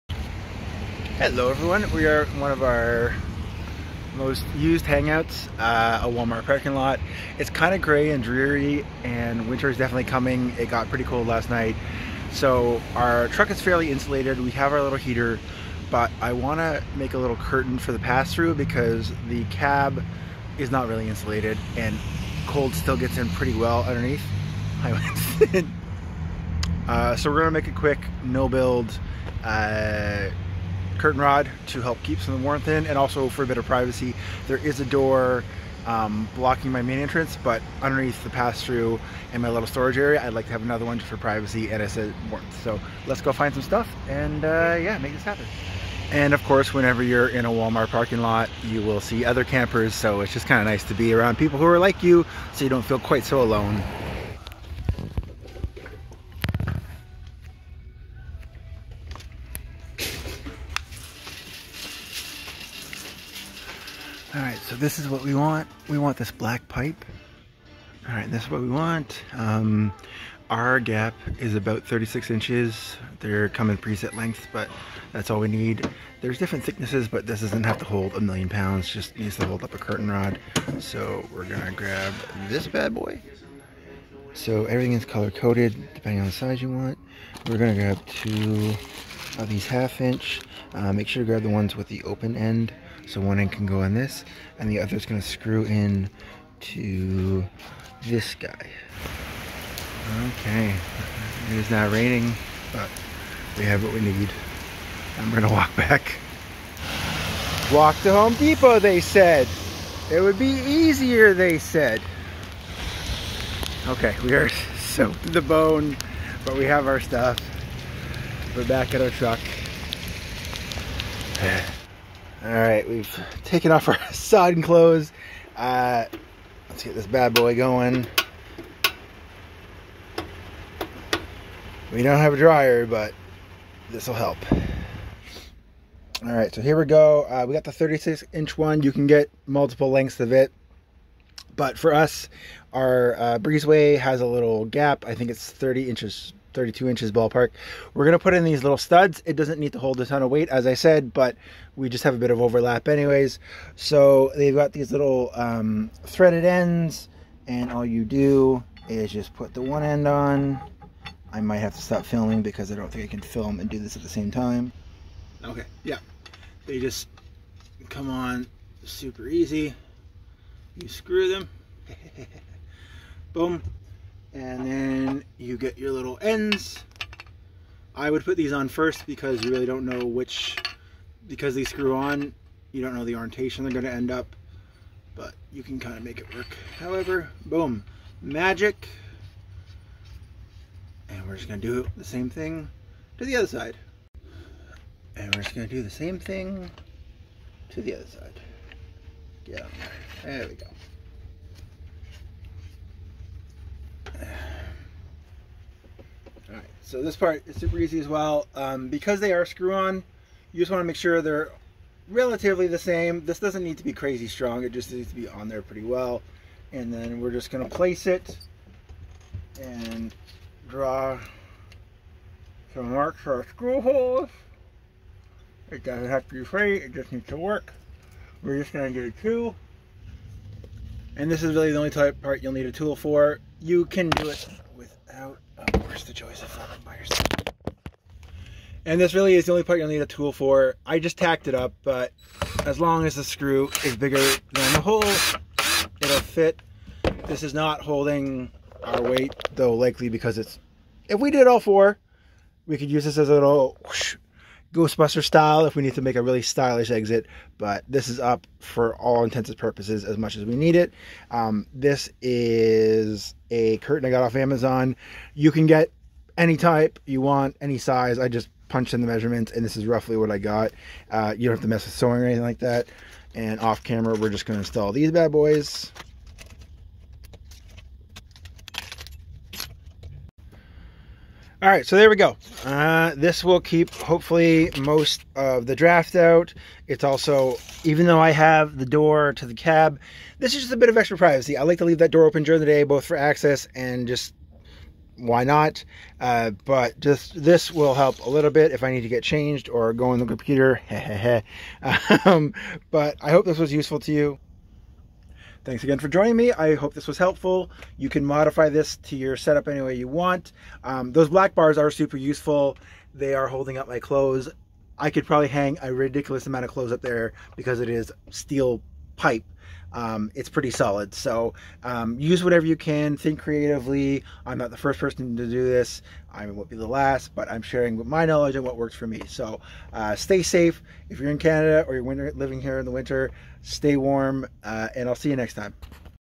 Easy "No Build" Privacy/Anti Draft Curtain for my Box Truck Camper Conversion
Winter is coming, and while this is insulated and we have heaters, a major source of cold getting is is through our little "secret passage" due to cold air coming in through the cab

So we do our best to find an easy solution to keep the cold out while getting a bit more privacy!

Bluetooth Thermal Shipping Label Printer